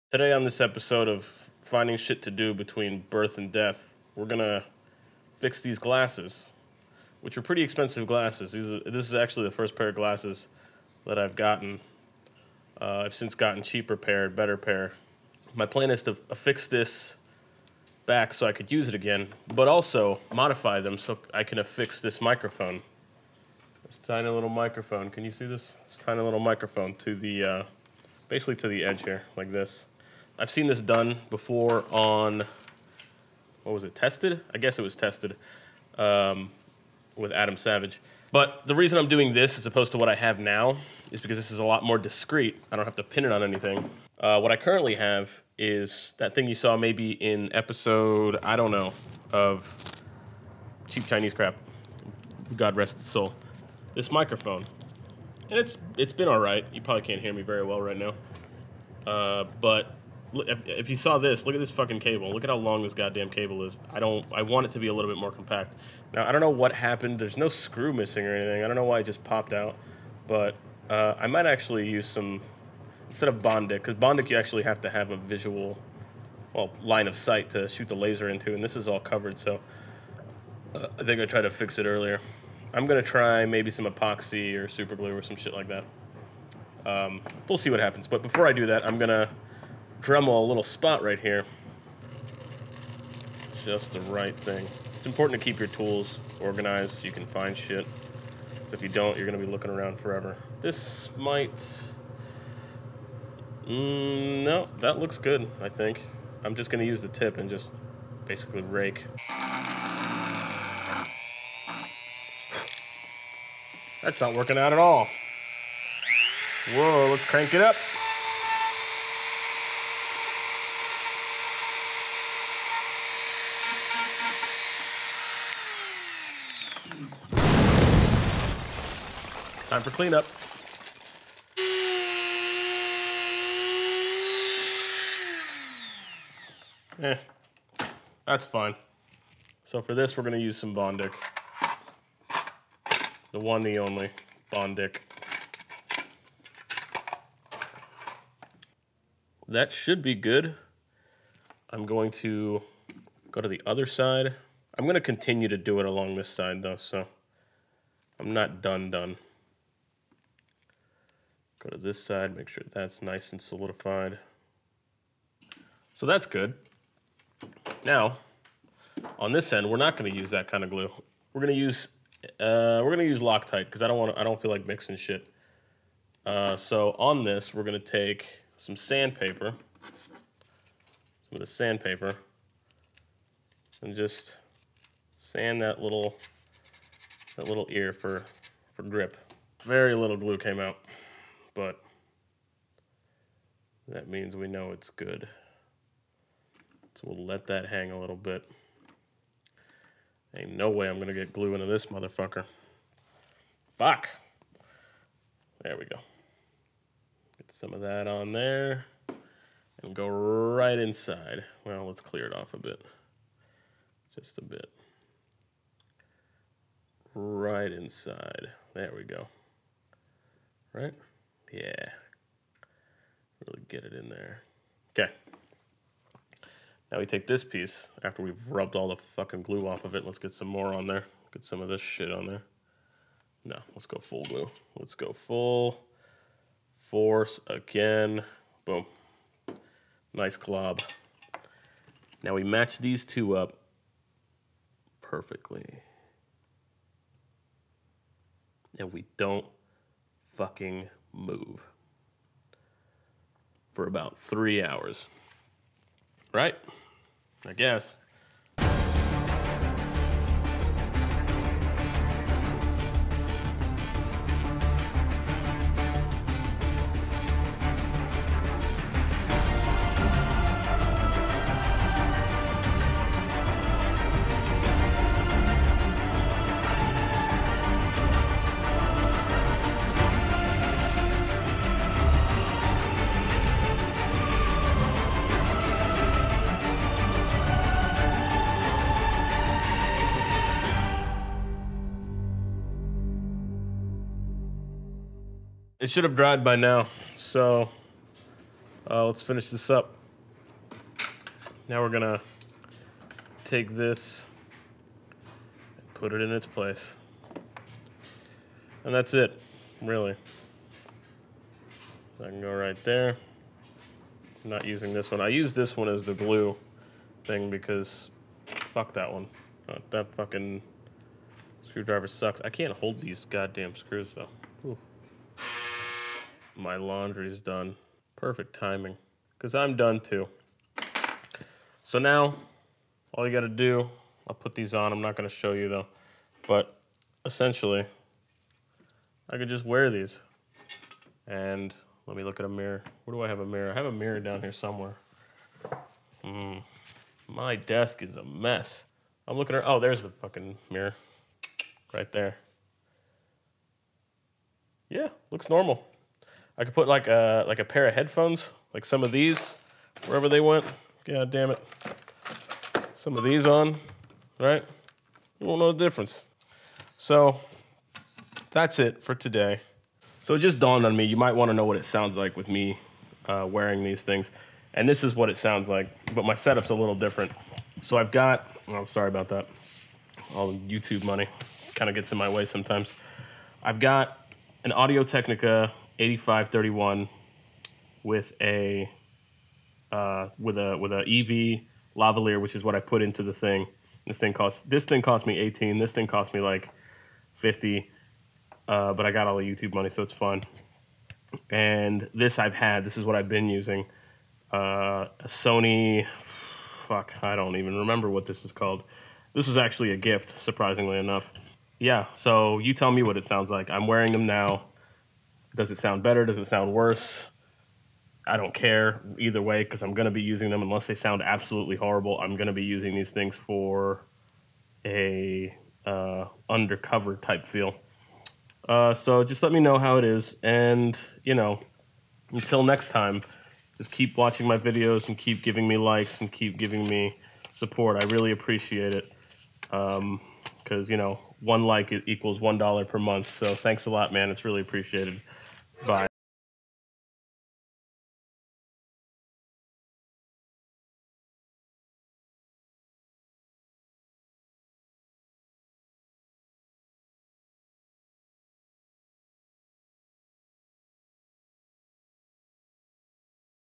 If It Ain't Fixed E10 - Microphone Glasses
Fixing a pair of glasses and also making a spy microphone.

1 like = $1 per month